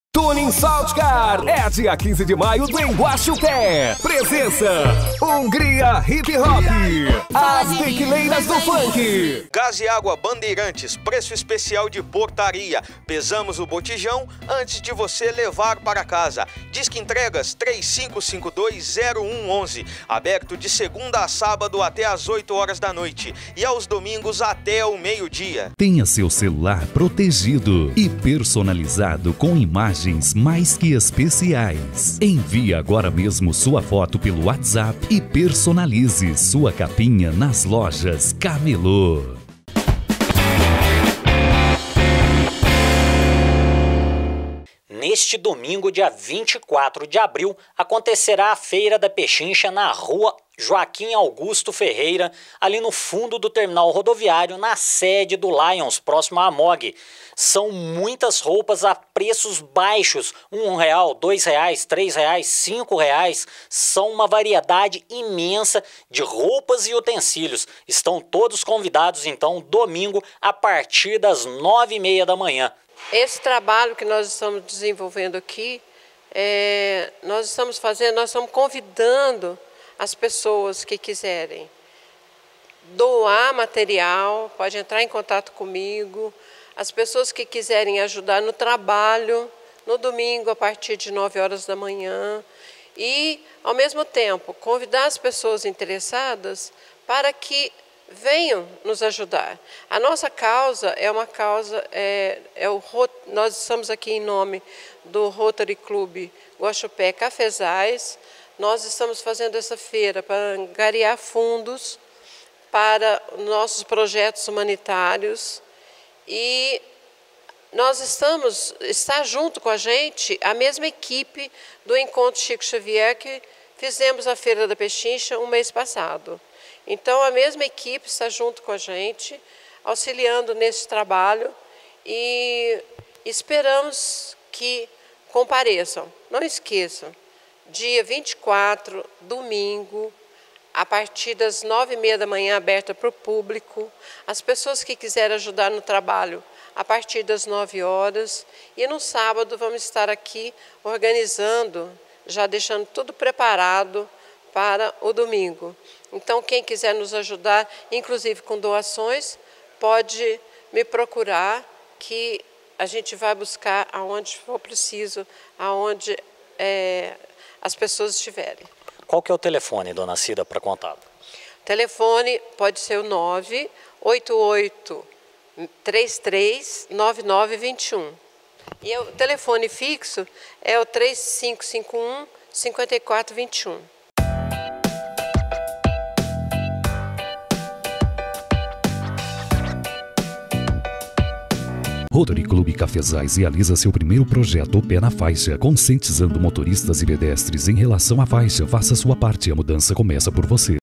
Acontecerá Feira da Pechincha na sede do Lions Clubes
Acontecerá neste domingo dia 24 de abril a partir das 9:30 horas da manhã, a feira da pechincha na Rua Joaquim Augusto Ferreira, nas proximidades do terminal rodoviário da cidade de Guaxupé (MG). São roupas e variedades com preços a partir de 1 real, dentre as variedades está um fogão e uma televisão. Na fique de fora desta oportunidade e venha participar e ajudar as entidades.